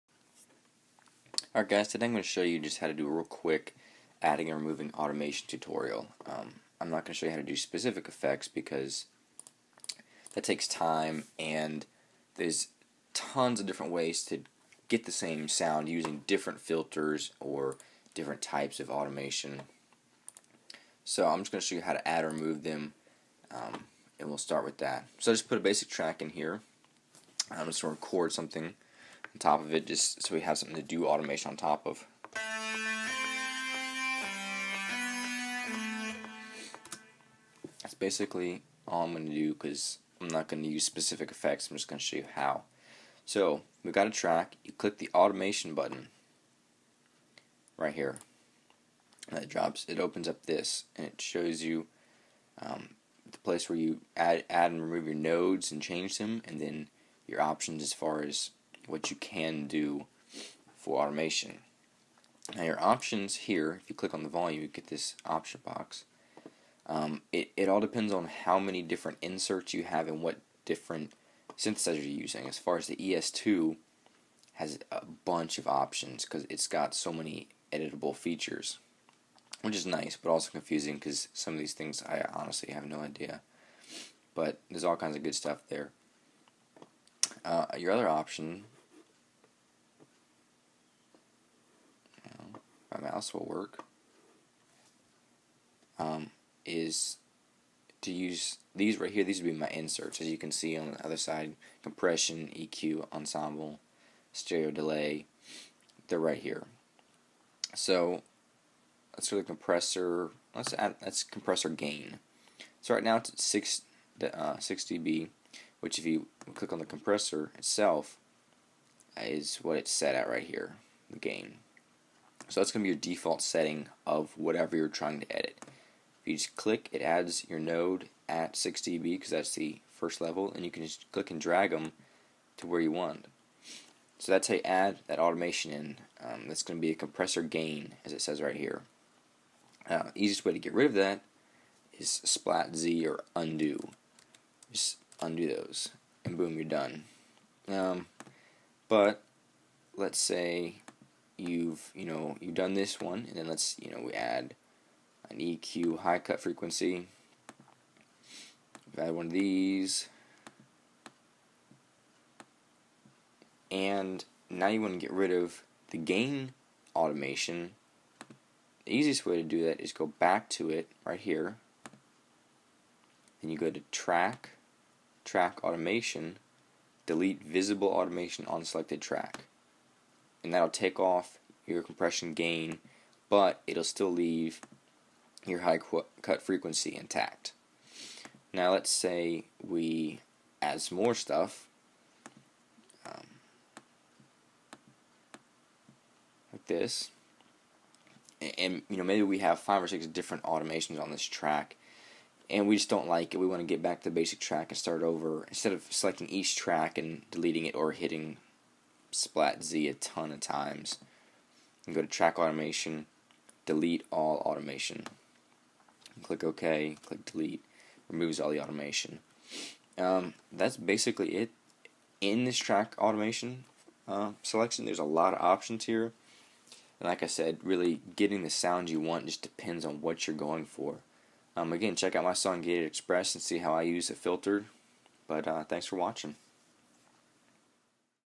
How to add and remove automation in Logic Pro
A quick tutorial on adding and removing automation in Logic Pro 9.